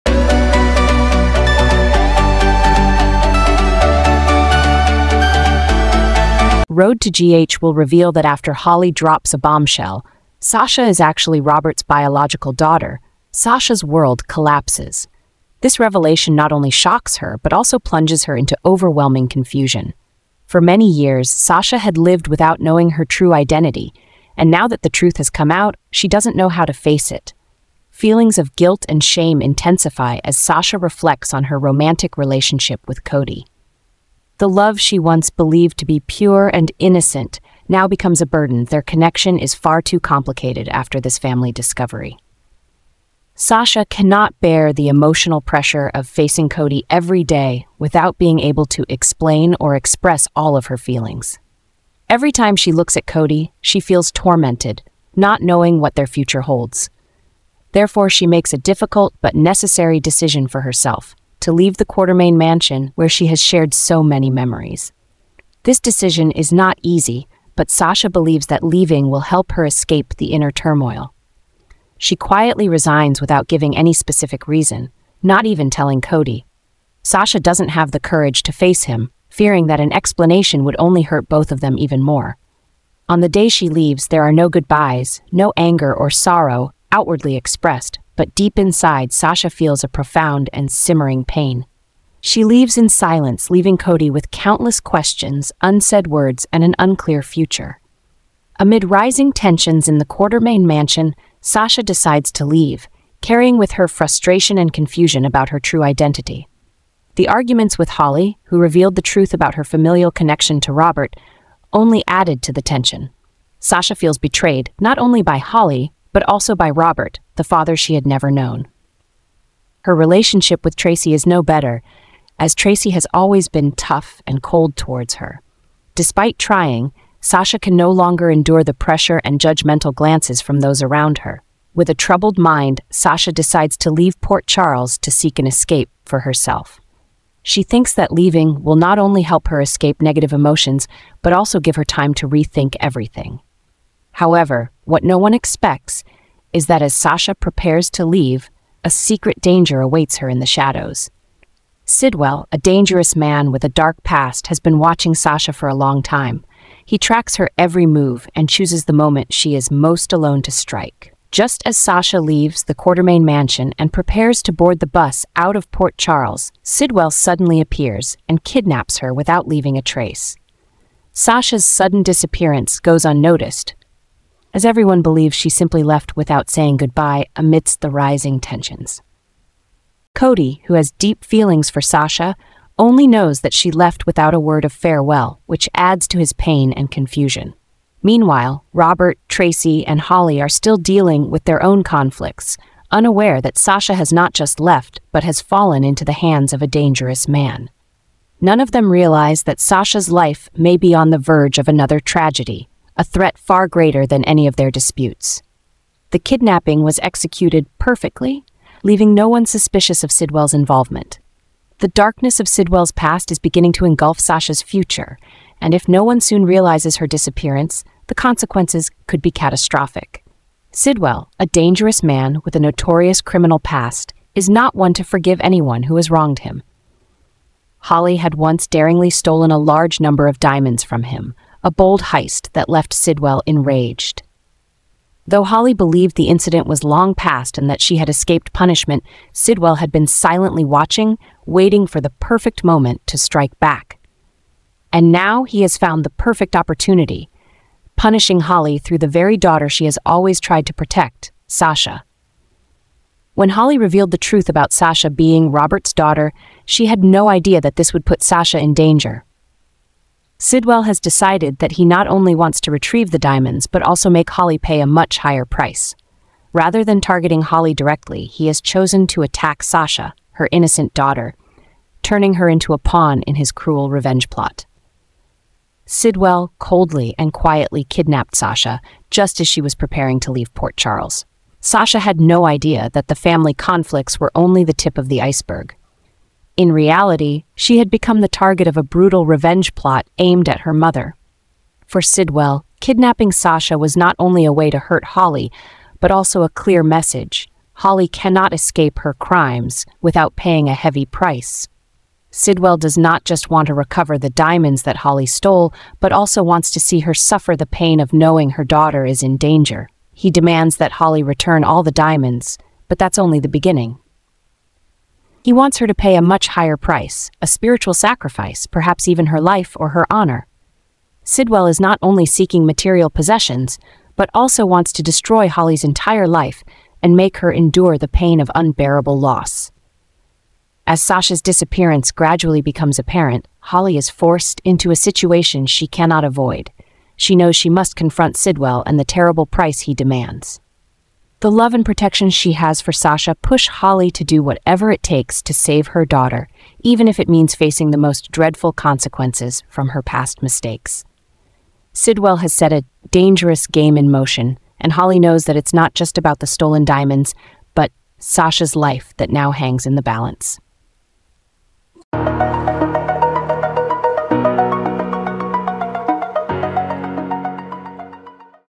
Sasha’s mysterious disappearance, Holly receives a threatening message | GH Spoilers
Sasha’s mysterious disappearance, Holly receives a threatening message